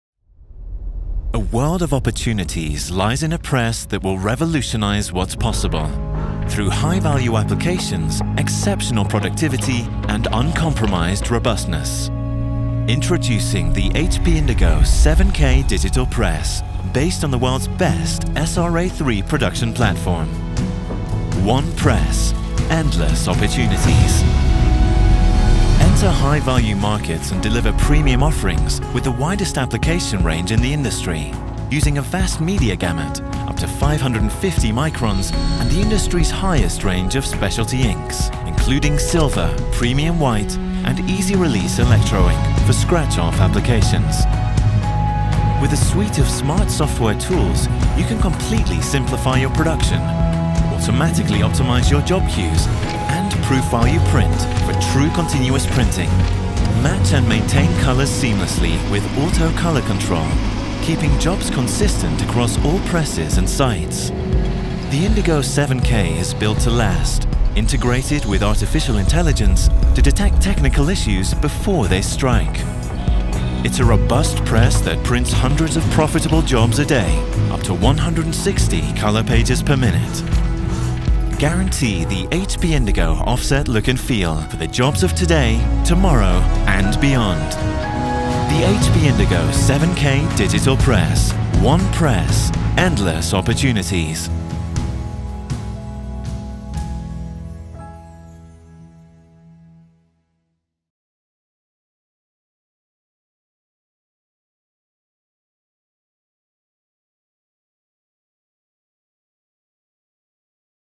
HP Indigo 7K Digital Press - One press, endless opportunities | Indigo Digital Presses
A world of opportunities lies in a press that will revolutionize what's possible, through high-value applications, exceptional productivity and uncompromised robustness.

Versatile digital presses with offset- and gravure- matching print quality for commercial and labels and packaging jobs